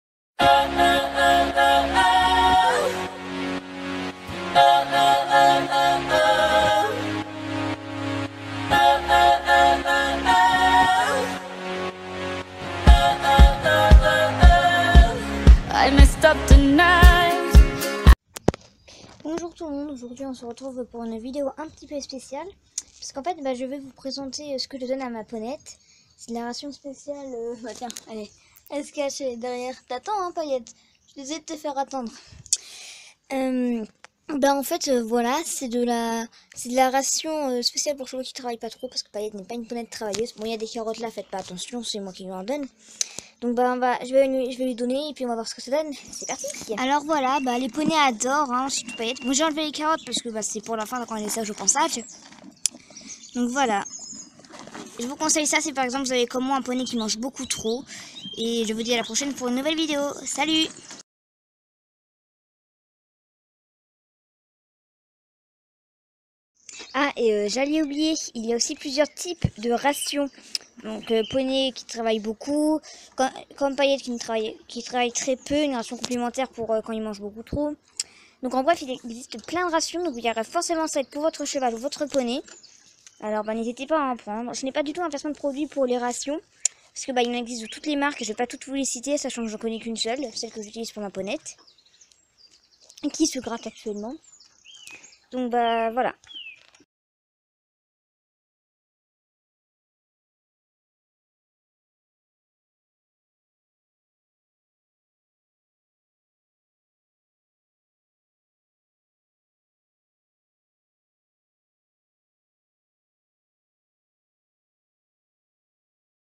Mon conseil #1 Les granulés, pour ou contre ?
Donc une vidéo particulière sur les granulés pour chevaux et poneys ;D
Mon blog :
Voili voilou à et sinon:
Je fait mes montages avec Windows Movie Maker
Je photographie avec un Wiko Rainbow 4G
Et c'est cette musique pour l'intro.